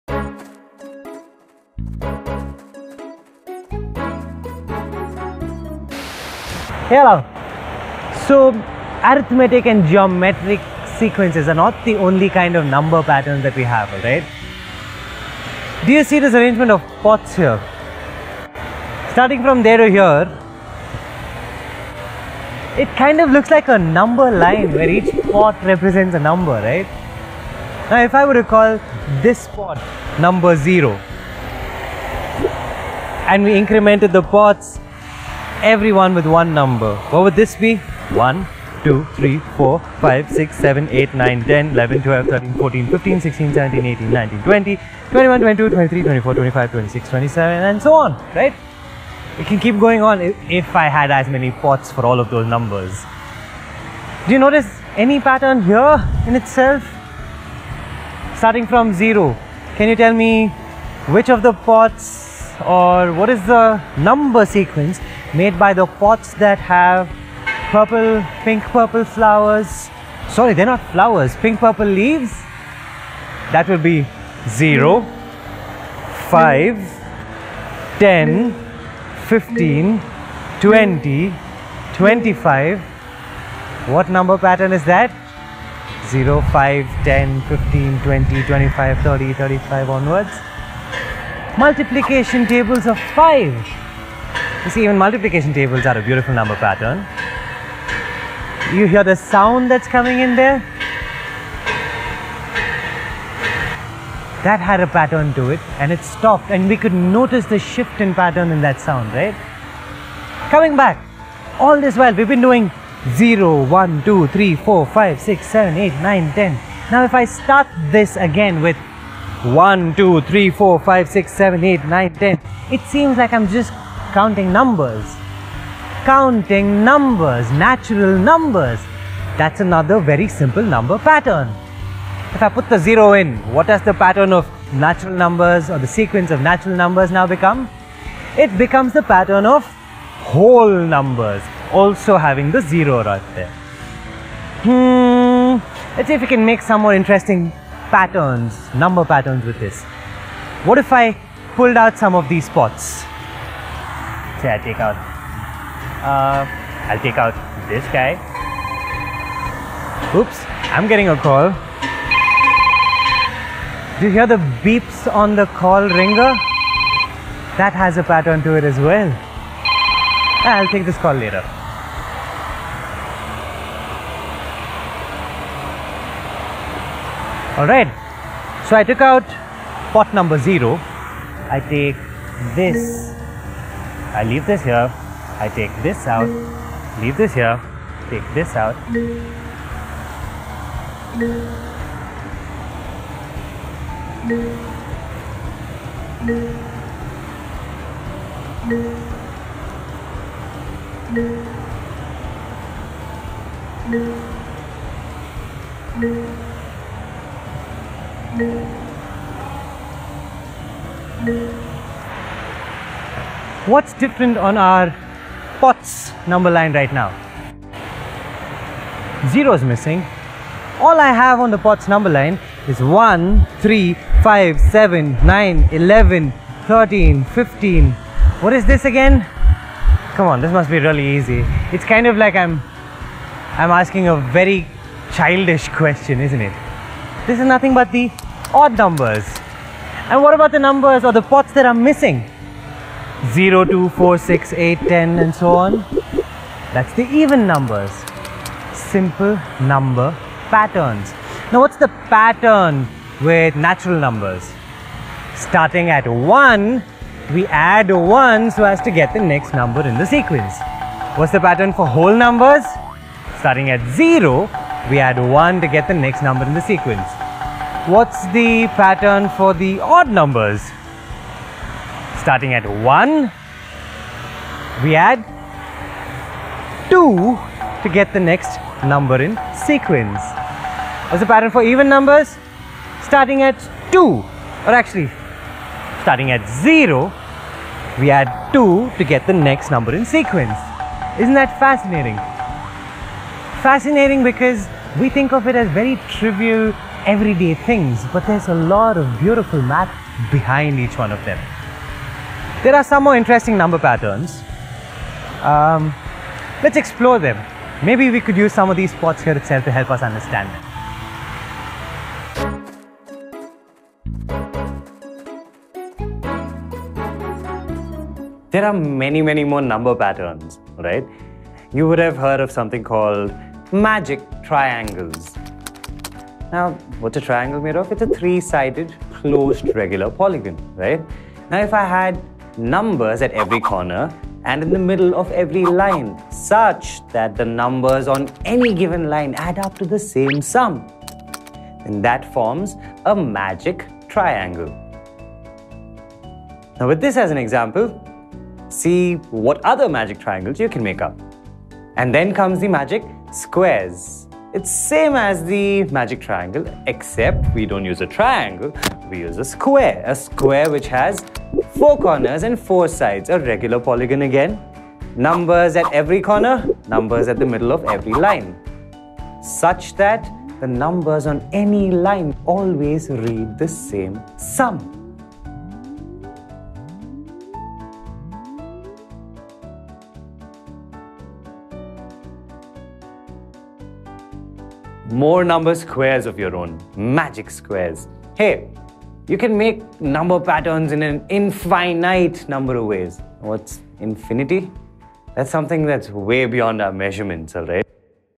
Number Patterns | Learn with BYJU'S
Numbers in mathematics has various patterns.  It is not just the arithmetic sequence and geometric sequence, but there are many more patterns to it. What is a magic triangle? What is a magic square? Heard of them before?

Watch this interesting video to discover a different number of patterns and see what magic triangles and squares are.

You can make different number patterns with natural numbers. The easiest way to form different number patterns is by placing all the natural numbers on a number line.

This video will show you simple ways to form different number patterns and how to classify numbers into odd numbers, even numbers, and other simple natural number patterns.

00:19 Number patterns made on number line
5:14  Magic Triangles & Magic Squares